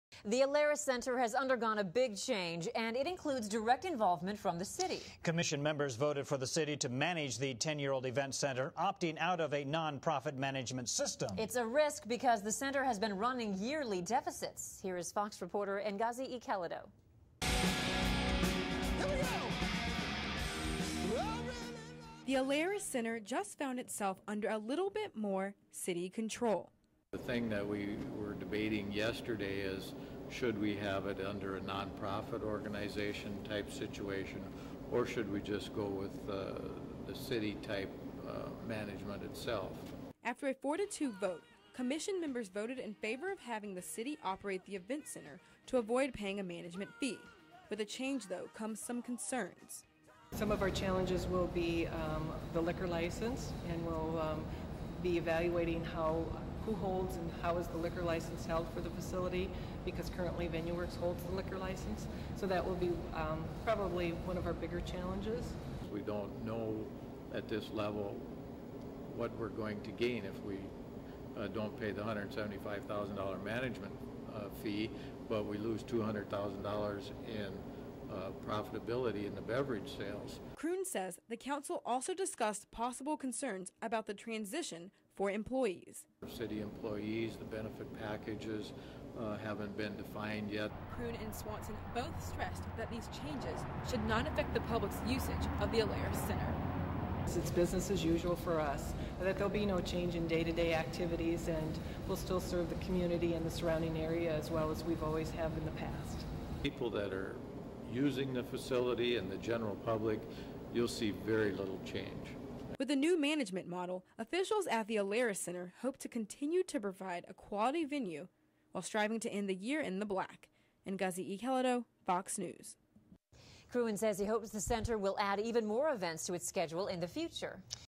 Alerus Center Changes
The Alerus Center has undergone a big change, and it includes direct involvement from the city.